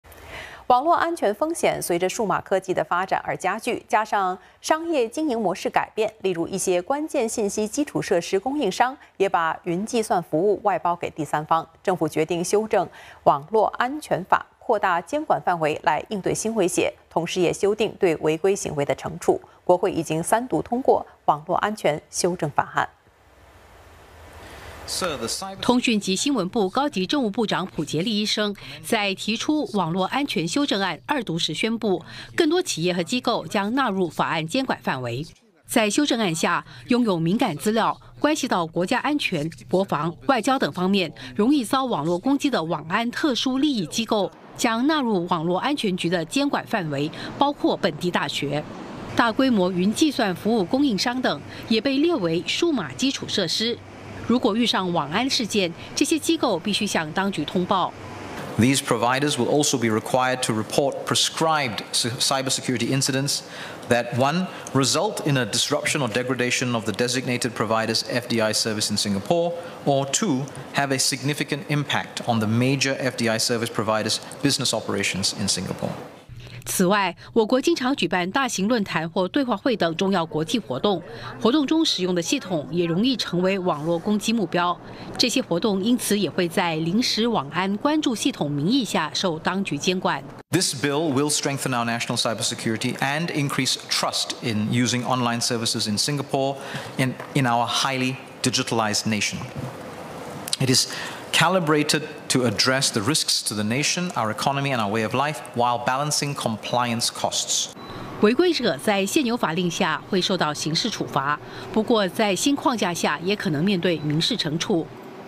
【国会】国会通过《网络安全修正法案》 重要机构若遇网袭须通报
国会通过《网络安全修正法案》。修订后的法令涵盖新的网络安全系统和实体，同时赋予网络安全局更大执法权力，以及修订对违规行为的处罚。